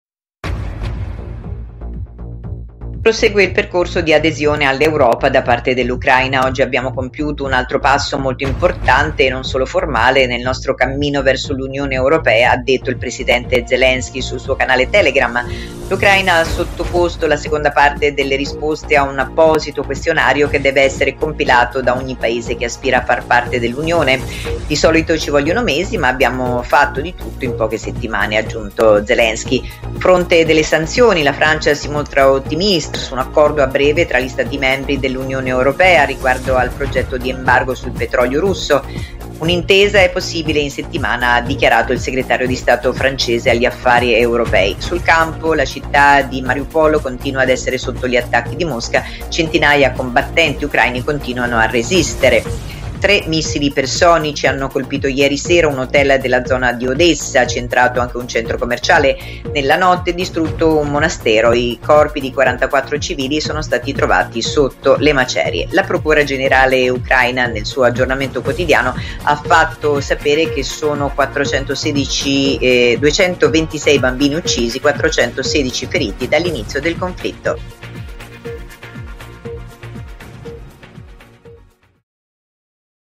Ucraina in Europa, Zelensky: “compiuto un altro passo importante”
Prosegue il percorso di adesione all’Europa da parte dell’Ucraina. “Oggi abbiamo compiuto un altro passo, molto importante e non solo formale, nel nostro cammino verso l’Unione europea” ha detto il presidente Zelensky sul suo canale Telegram.